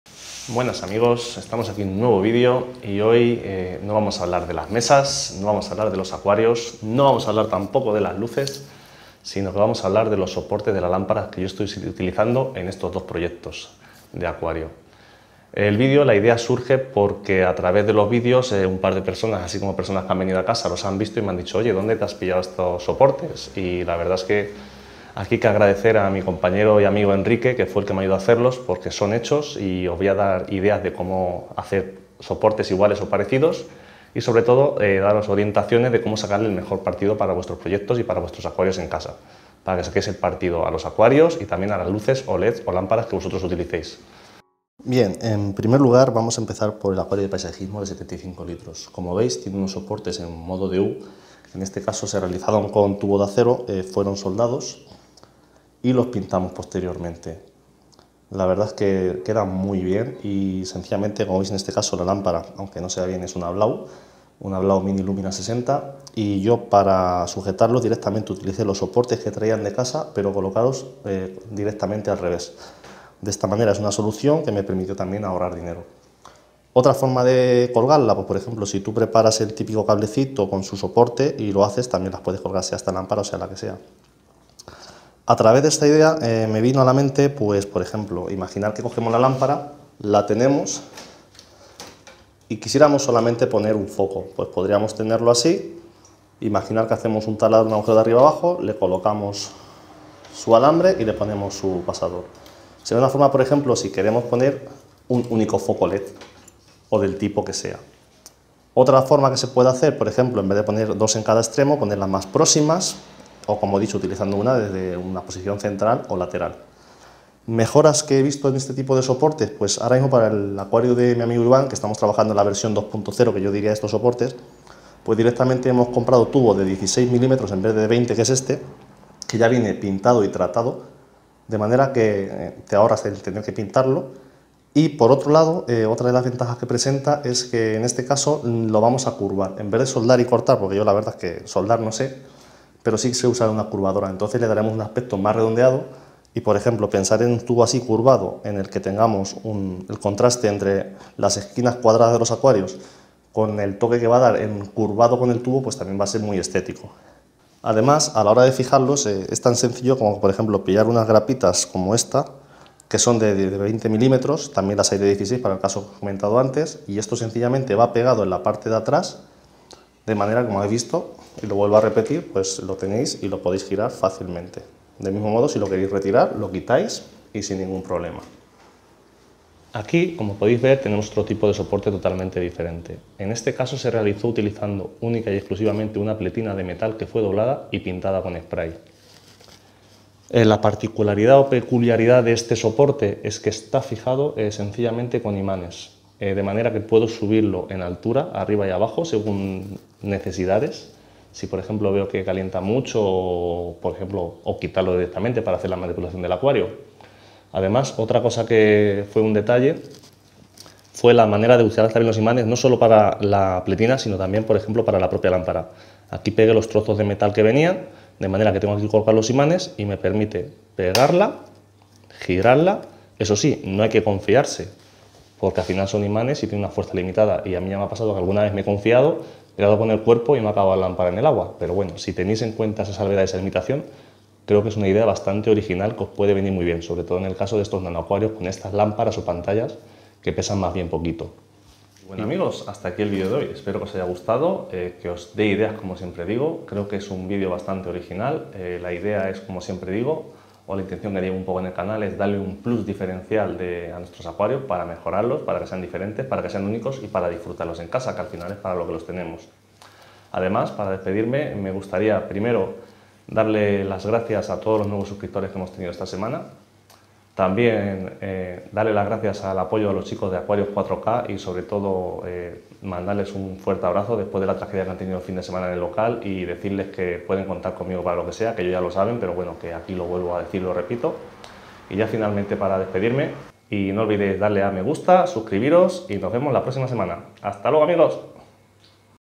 Cómo Hacer un Soporte Casero para Lámparas LED de tu Acuario - DIY
En este brico vídeo, os muestro los soportes para lampara que hemos creado. Para las lamparas LED de 2 acuarios. Agradecer a mi amigo Enrique, por su colaboración y ayuda al proyecto.